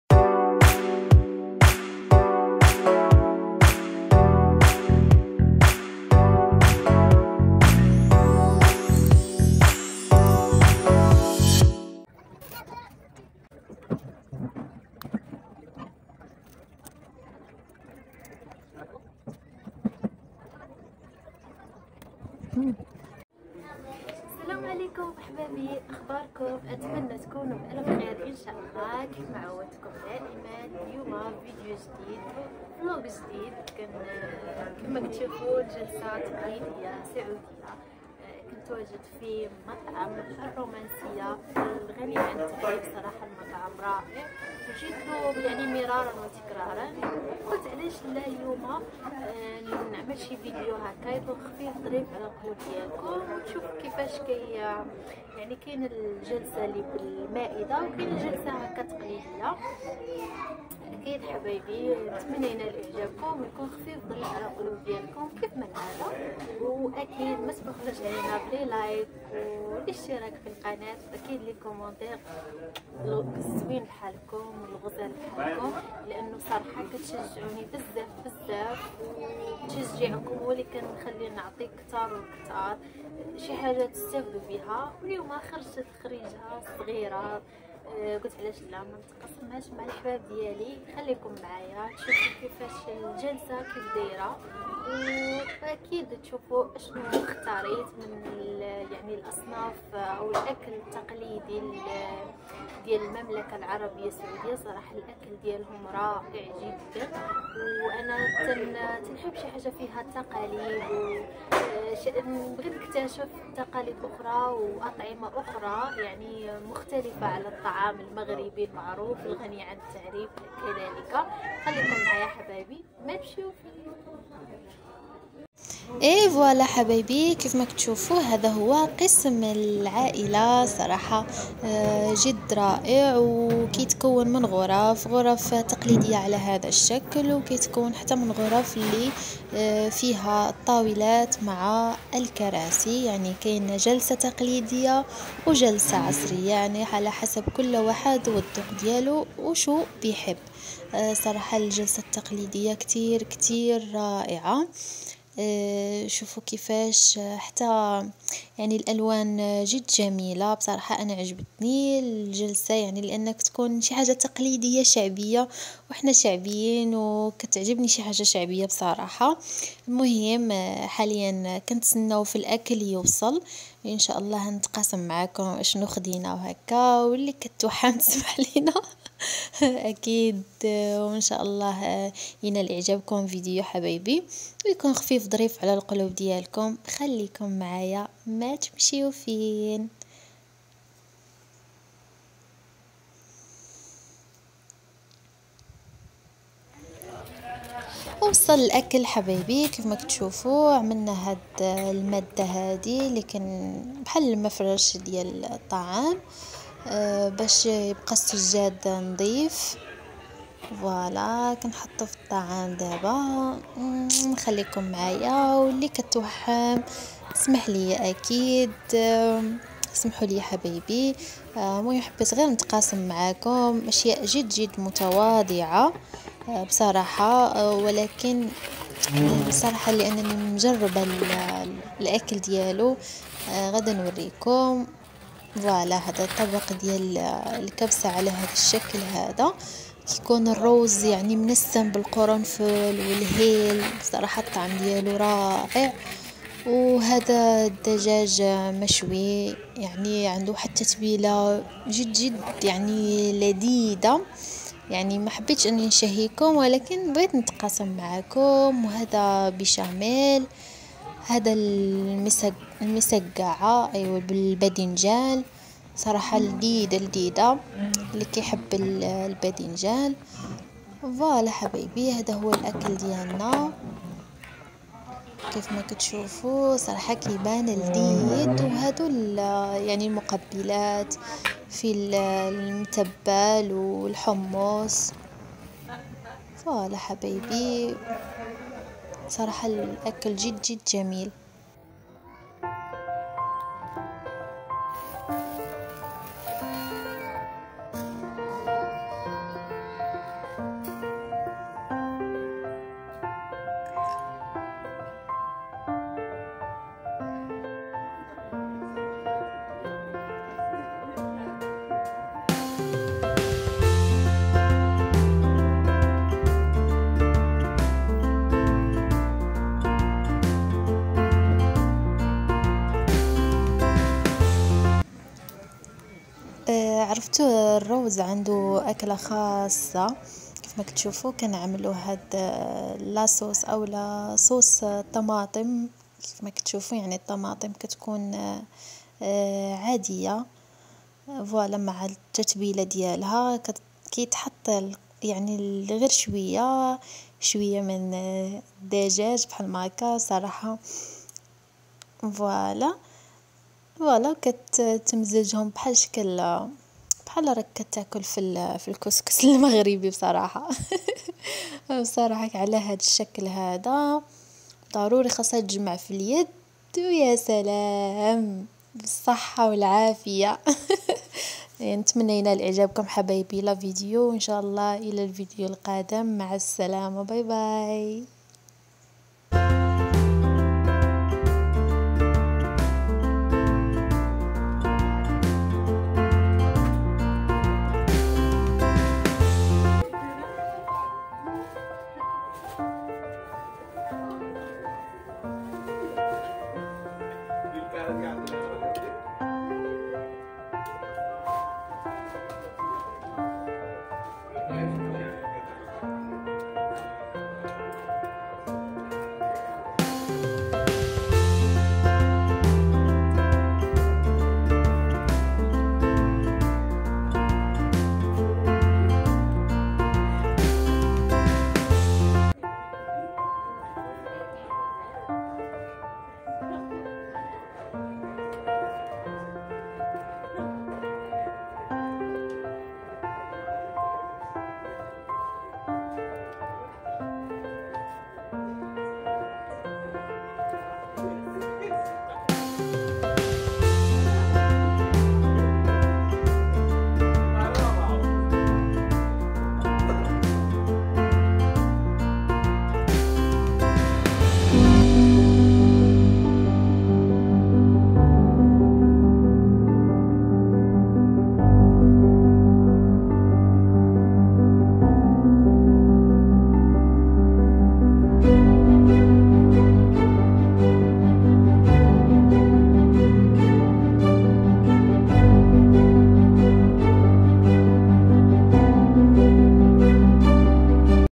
عزومة عشاء متواضعة في مطعم الرومانسية
عزومة عشاء متواضعة في مطعم الرمانسية
شاركت معاكم عشاء شعبي تقليدي متواضع اكتر من رائع